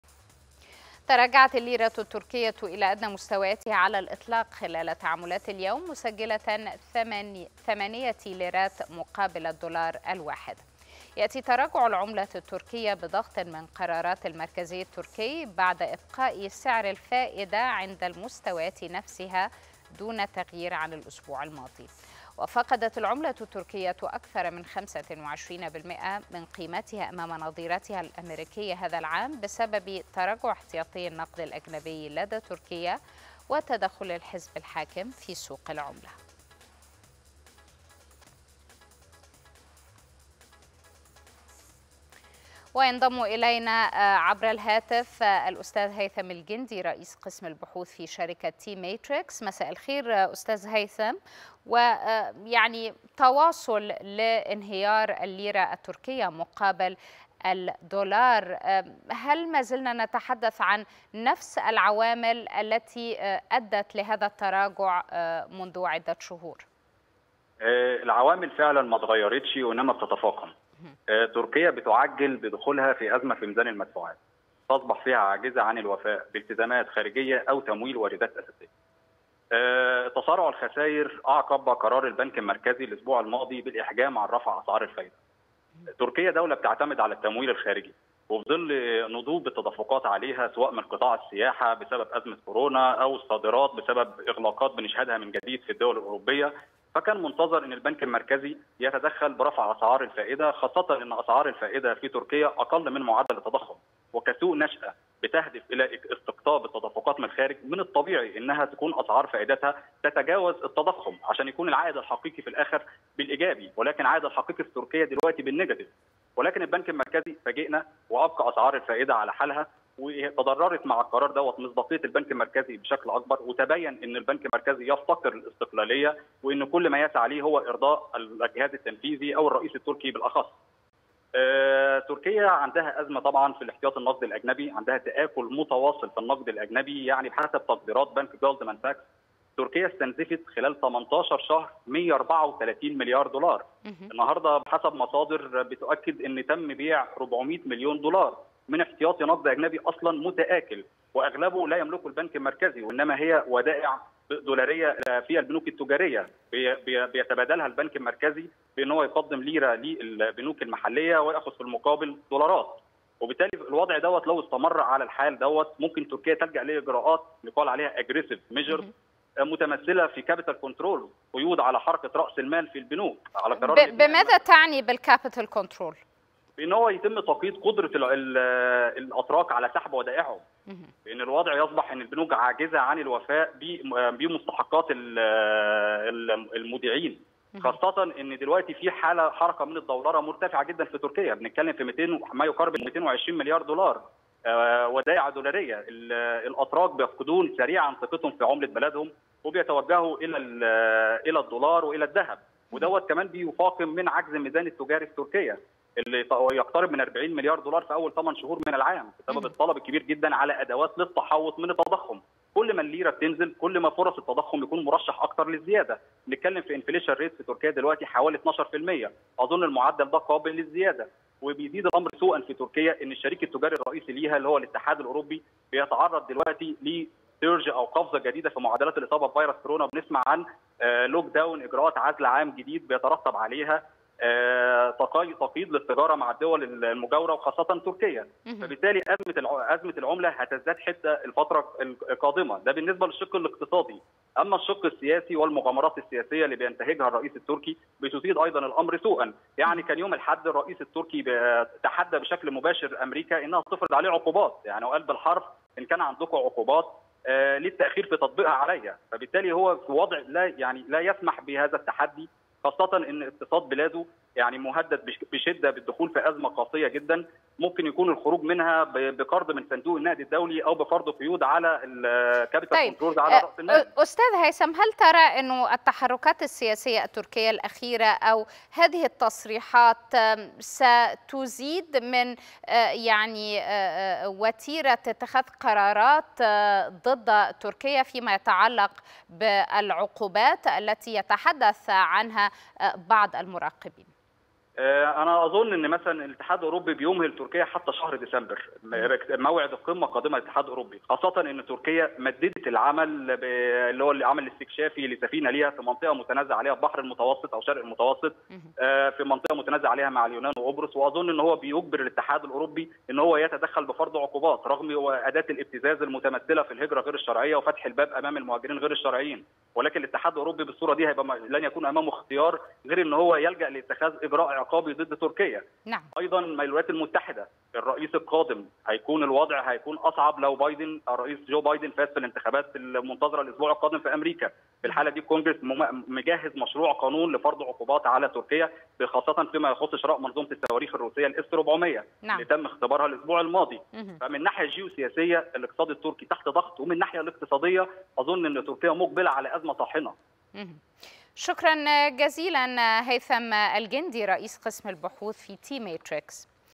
مال وأعمال | الليرة التركية تتراجع إلى أدنى مستوياتها أمام الدولار.. اعرف التفاصيل
مال وأعمال | الليرة التركية تتراجع إلى أدنى مستوياتها أمام الدولار.. أعرف التفاصيل
26 أكتوبر  2020